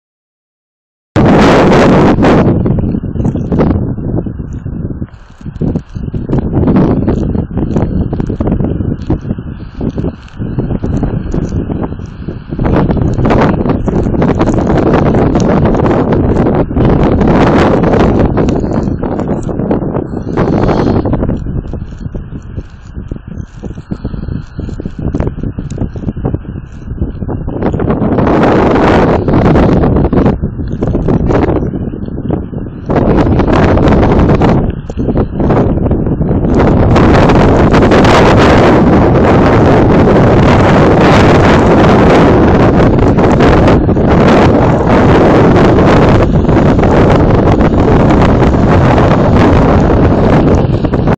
27 Febrero 18:58 horas,empezando a Nevar en Sierra Espuña.
18 h.58 minutos,nevando en Sierra Espuña.